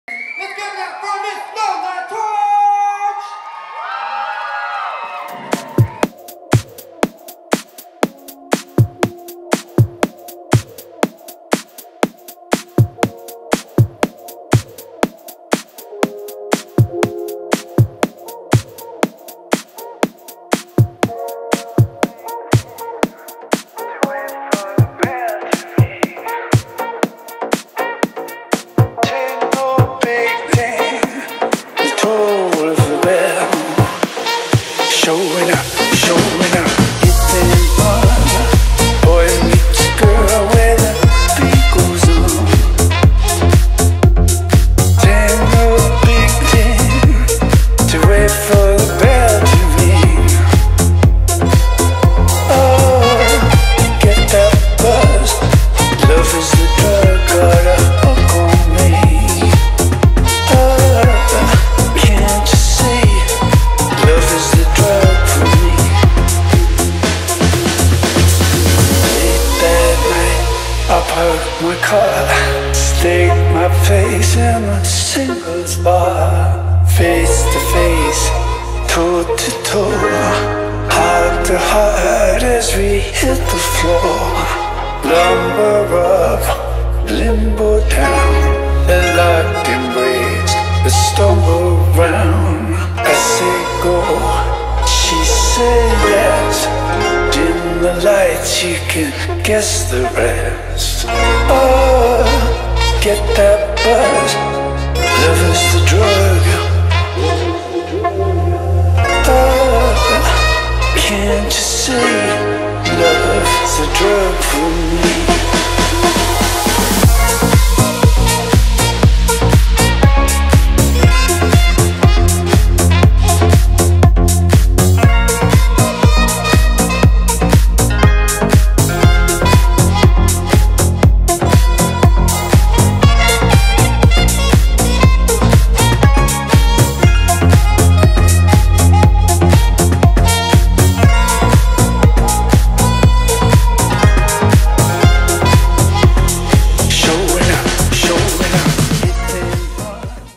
Lola Torch Highlight Reel
Presenting... Lola Torch!
Highlights and stills from past performances.